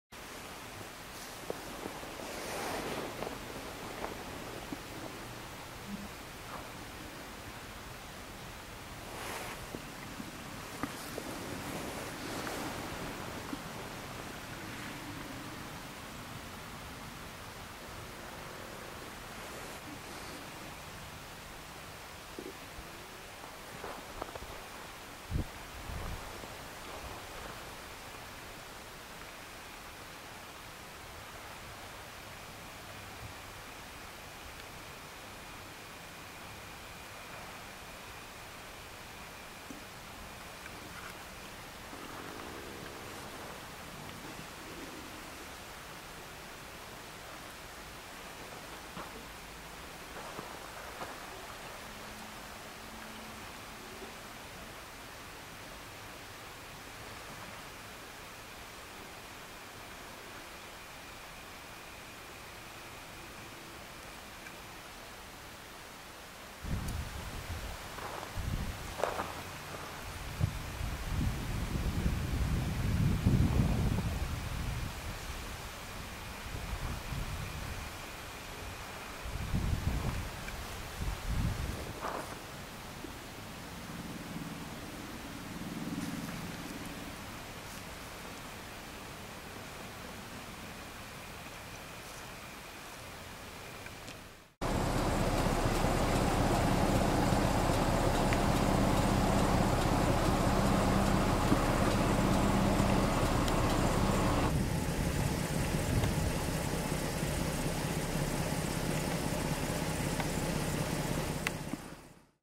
Sunset over the Ice and an Owl
The ice is building against Tawas Point. It's beautiful and dangerous. It's getting hard to tell where the shoreline really is. I got lucky when an owl flew in front of me and landed on a branch as I was leaving.